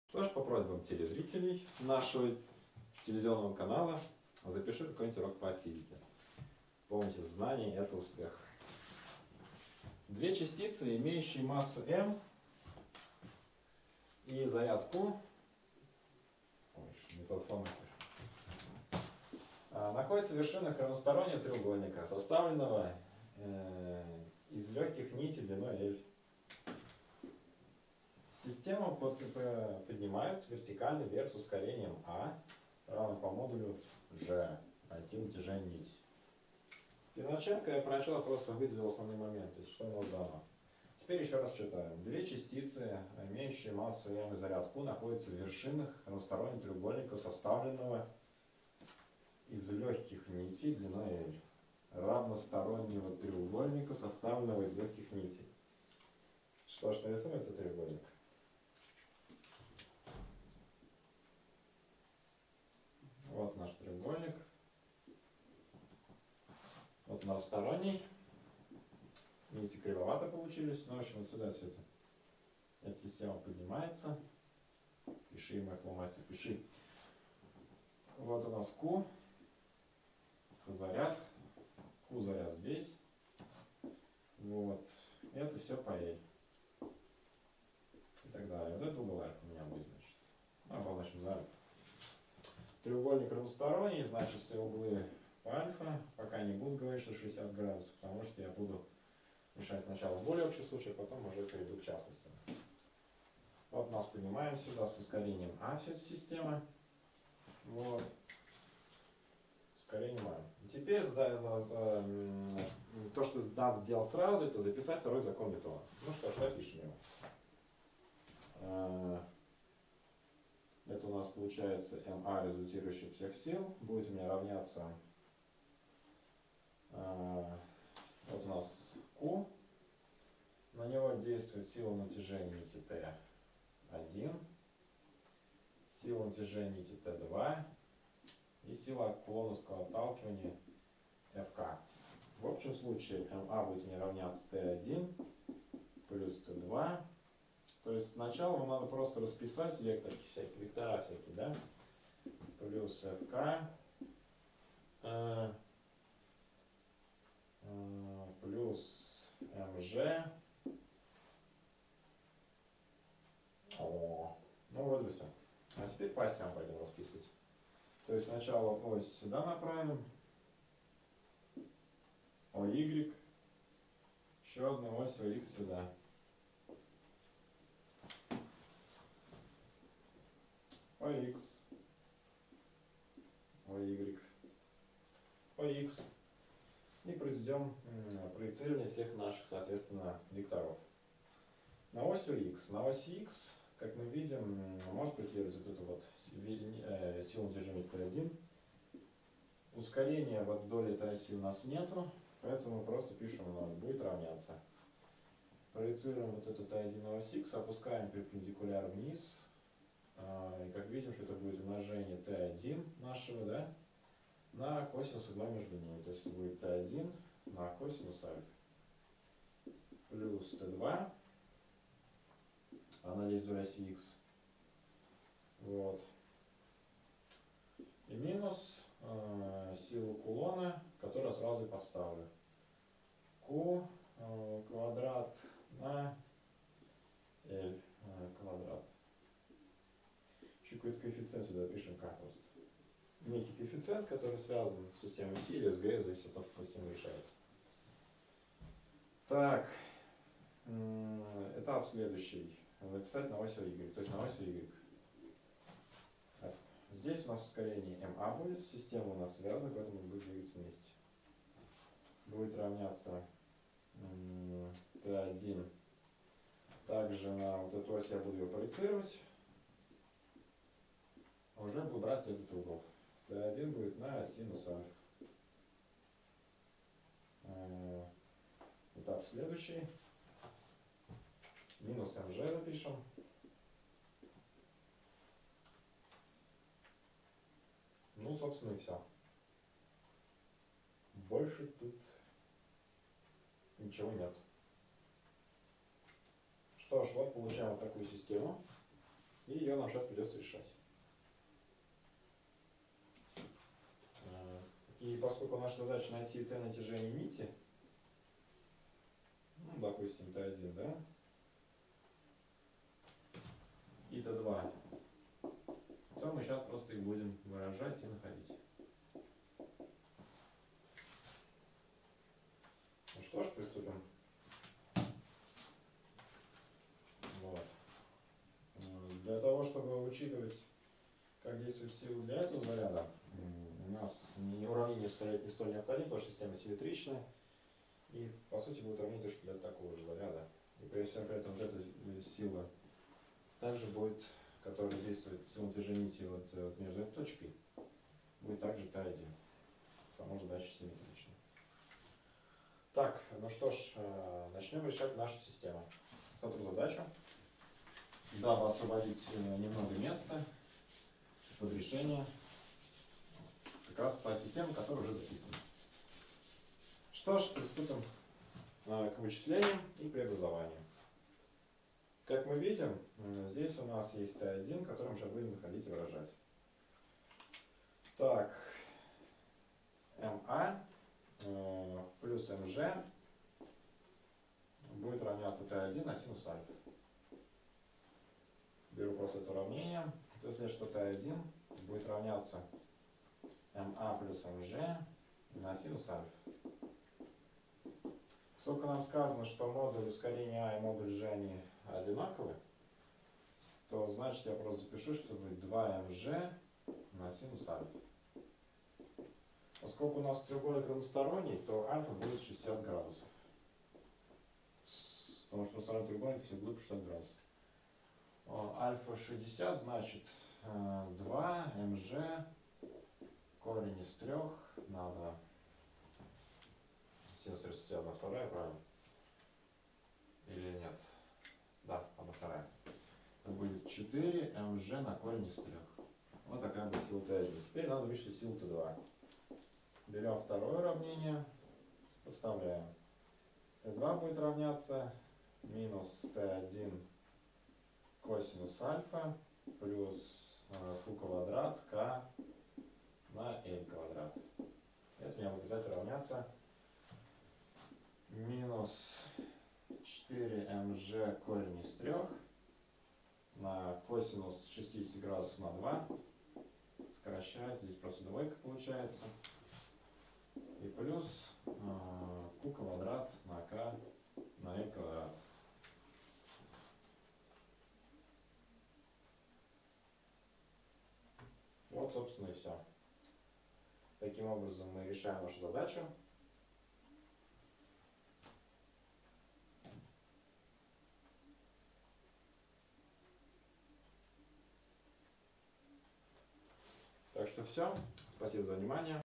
Электрическое поле.Закон ньютона
Две частицы, имеющие массу m и заряд q, находятся в вершинах равностороннего треугольника, составленного из лёгких нитей длиной l. Систему поднимают вертикально вверх с ускорением а, равным по модулю g. Определите натяжение нити, соединяющей частицы.